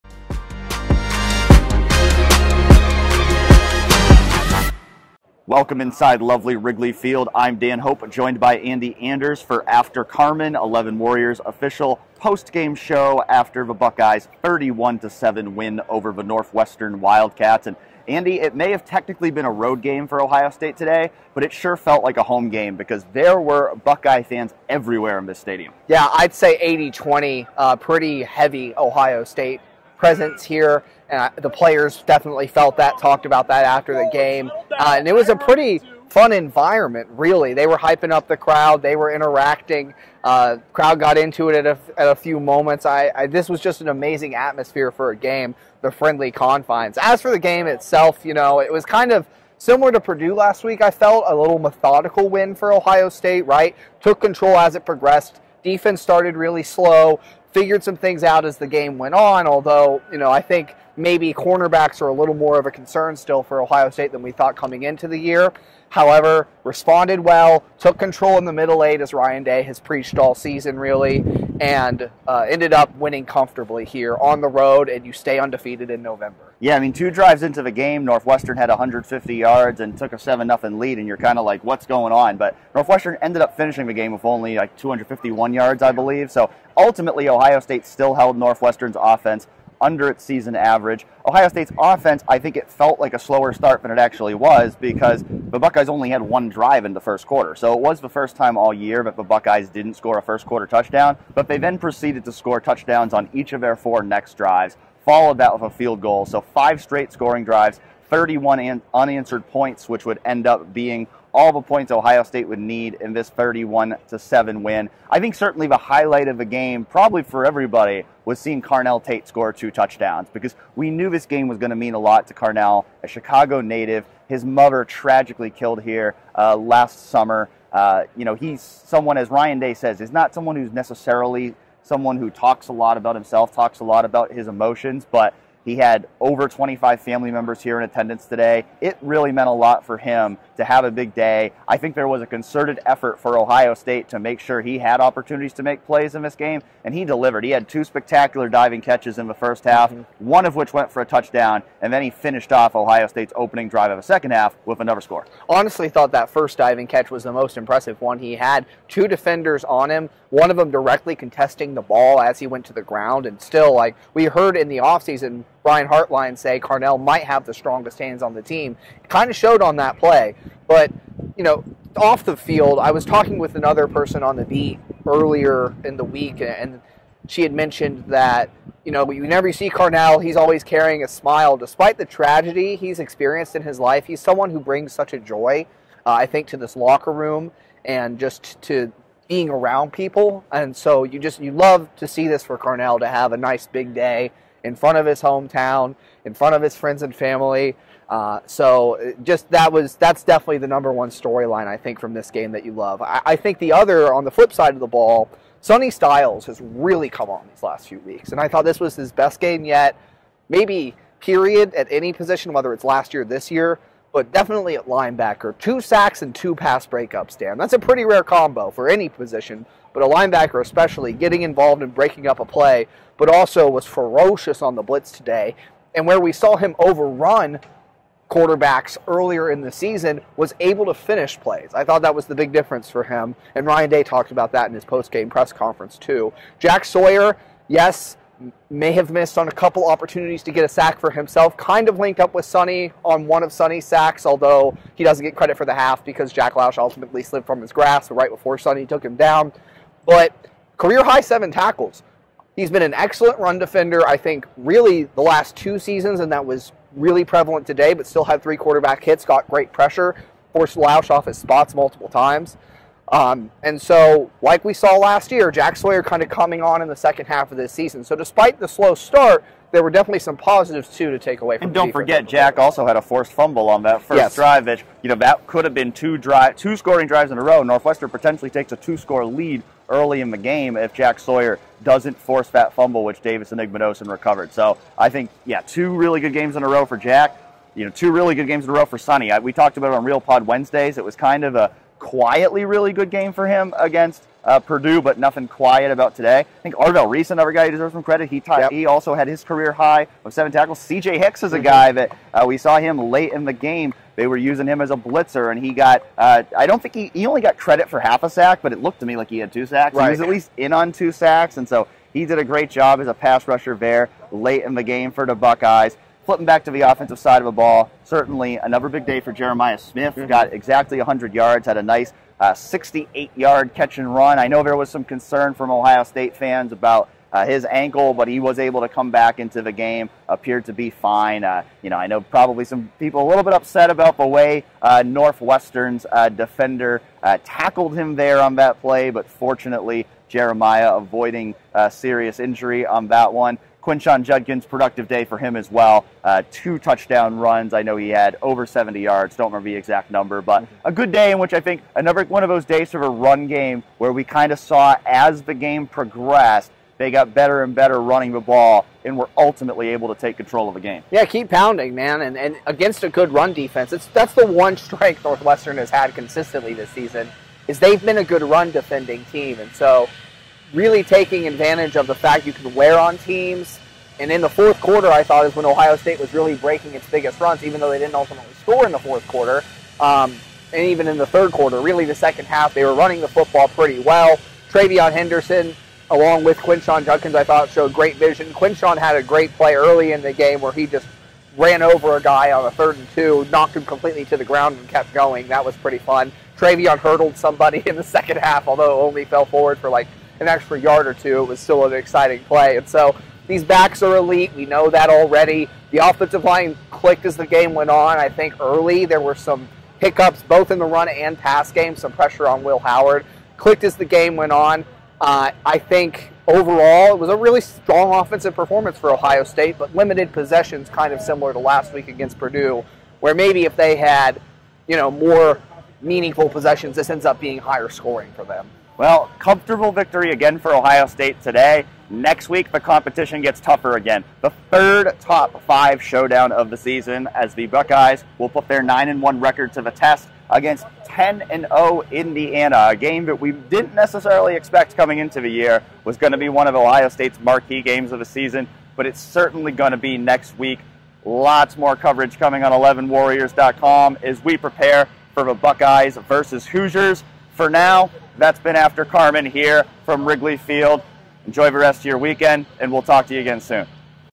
After Carmen: Carnell Tate, Sonny Styles stand out as Buckeye fans make trip to Wrigley a home game
Carnell Tate and Sonny Styles were the biggest stars of the day as Ohio State picked up another methodical win over Northwestern, 31-7, in front of a Wrigley Field crowd that was at least 80% Ohio State fans, giving the Buckeyes a home crowd in what was technically a road game.

Dan Hope and Andy Anders discuss all of that and more on After Carmen, Eleven Warriors’ official postgame show.

Head to Eleven Warriors to stay up to date on Ohio State football, basketball, recruiting, and other news:

Follow 11W on social media!